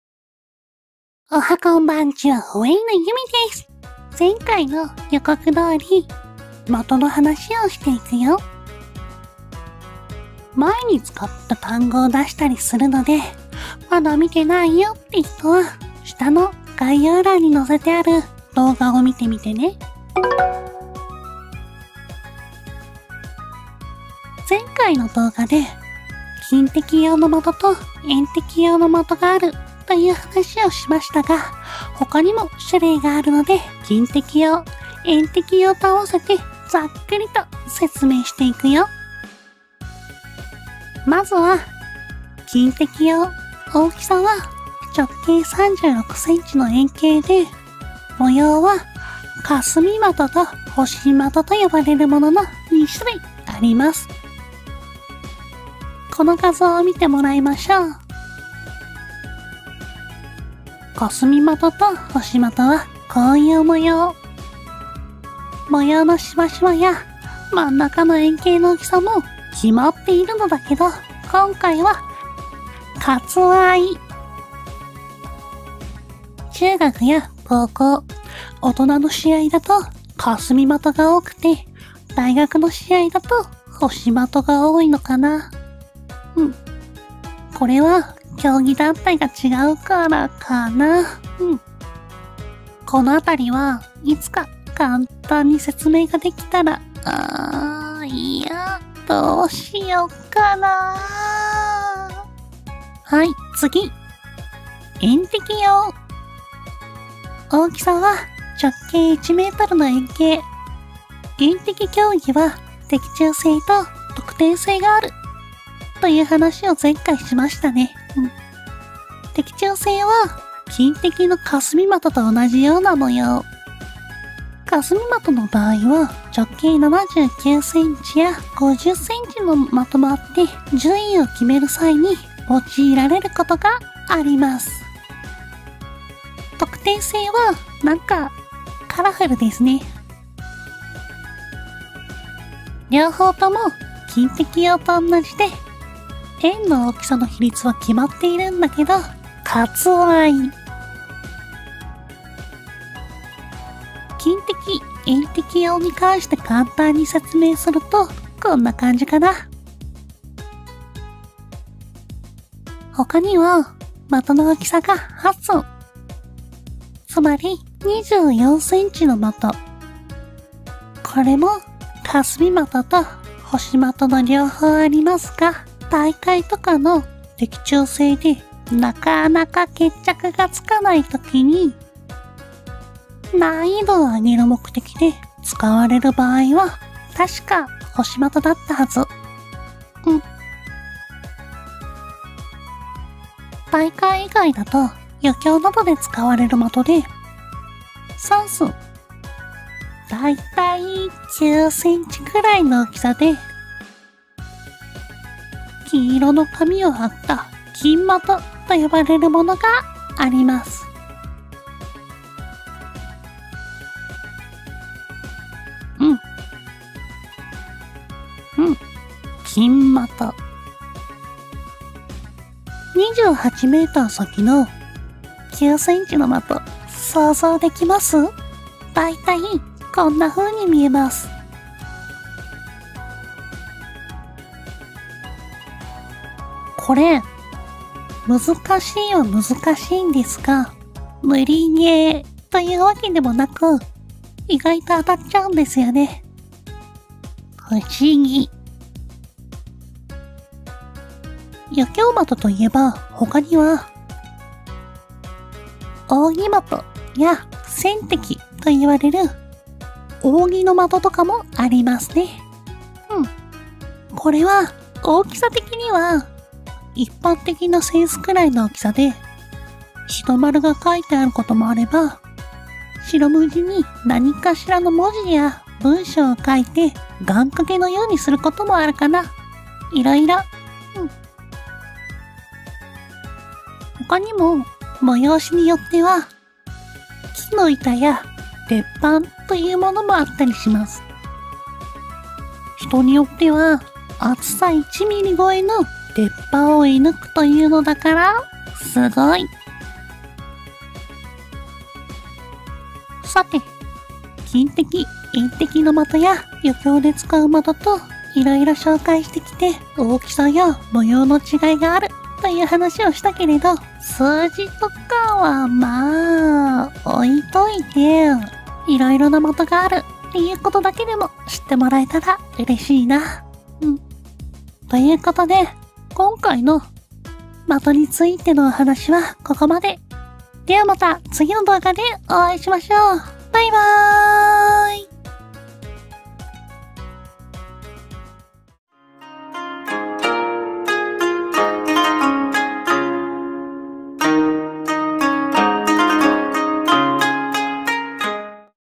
#09 【 弓道 】VTuberが的について説明するよ【 VTuber 】
弓道経験者には当たり前だけど
一般的にはあまり知られていなそうな話

〇英語字幕協力
　Tsundere Ojisan (Kai)氏